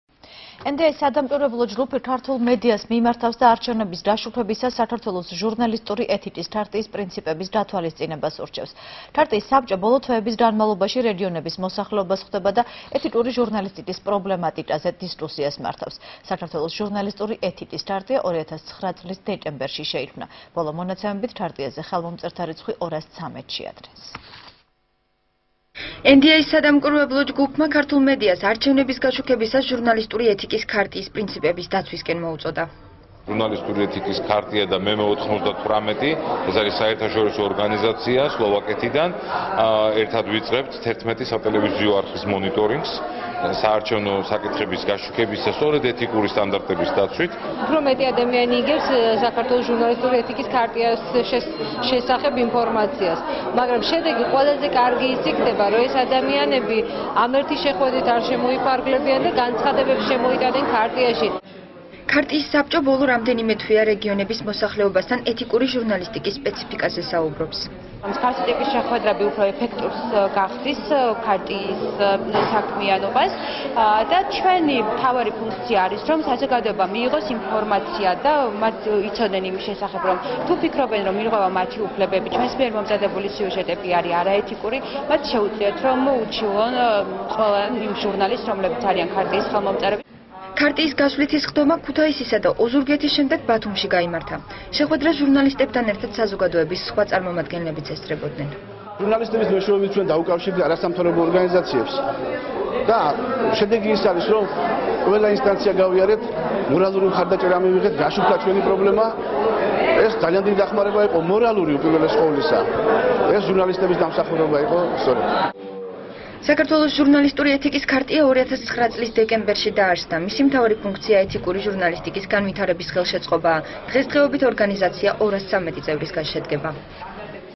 ახალი 9 | NDI-ს მოწოდება | 30.06.12
ენდიას სადამკვირვებლო ჯგუფი ქართულ მედიას მიმართავს და არჩევნების გაშუქებისას საქართველოს ჟურნალისტური ეთიკის ქარტიის პრინციპების გათვალისწინებას ურჩევს.  ქარტიის საბჭო ბოლო თვეების განმავლობაში რეგიონების მოსახლეობას ხვდება და ეთიკური ჟურნალისტიკის პრობლემატიკაზე დისკუსიას მართავს. საქართველოს ჟურნალისტური ეთიკის ქარტია 2009 წლის დეკემბერში შეიქმნა. ბოლო მონაცემებით, ქარტიაზე ხელმომწერთა რიცხვი 213-ს შეადგენს.